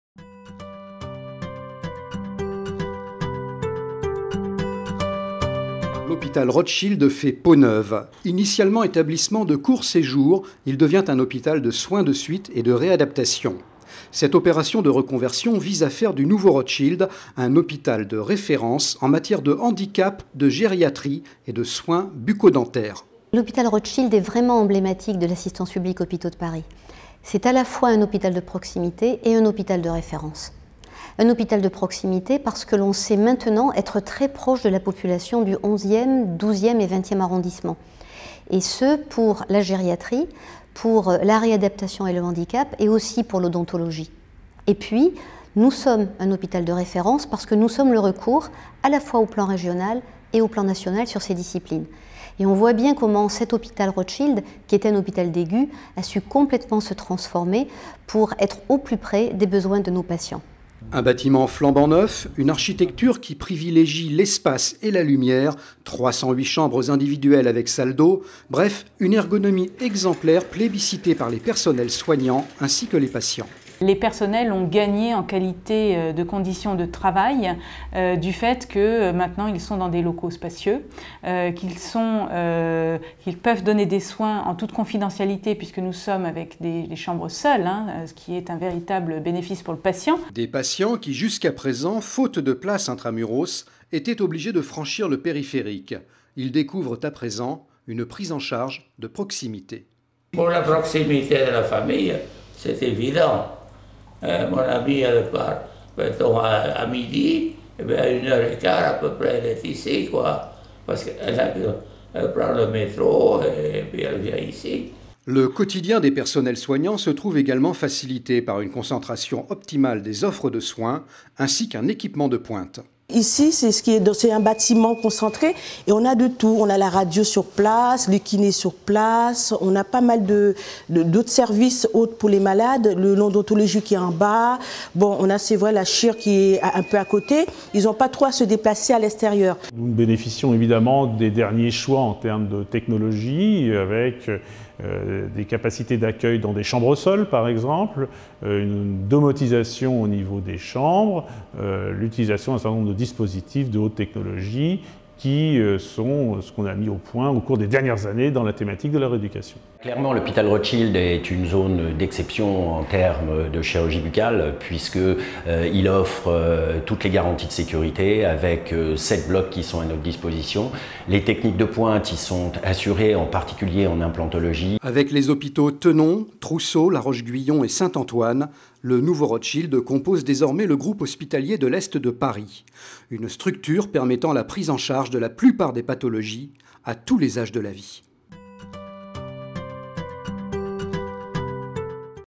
Le nouveau Rothschild
Juillet 2011. Inauguration du "Nouveau Rothschild". L'hôpital Rothschild a fait peau neuve et se positionne comme hôpital de reference de l'est parisien...